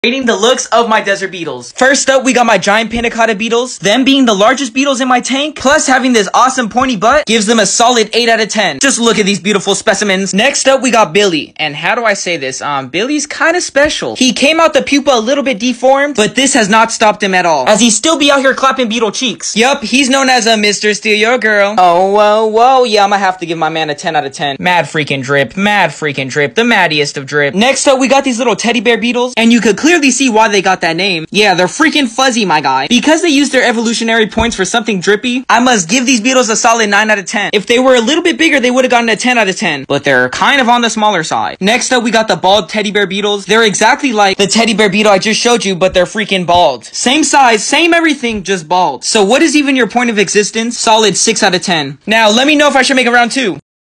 Fit Check beetle edition!!!!!!!🔥💧
in this beetle collection video I show off the different beetle species that I keep and rate their looks in a comedic way!